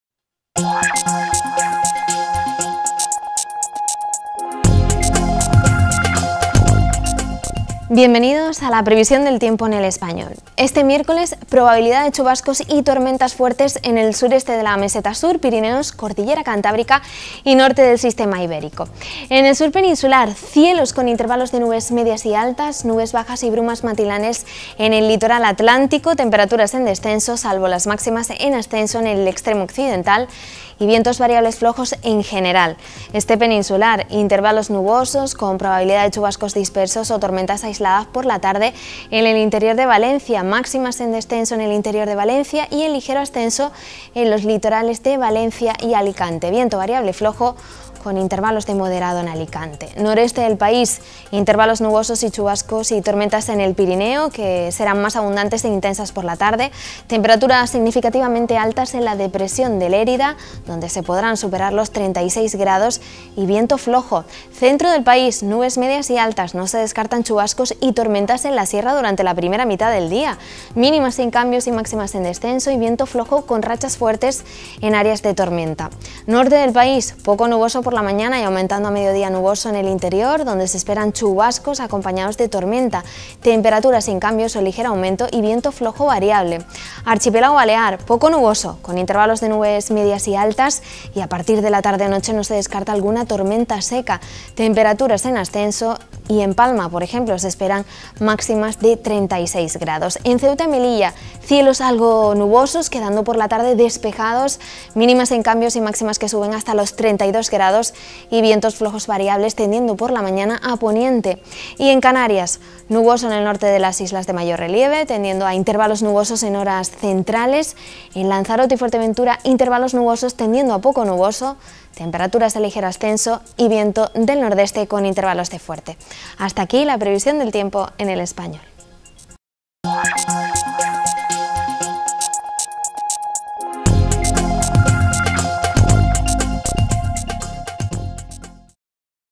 Tiempo EL ESPAÑOL
La previsión del tiempo del miércoles 22 de julio.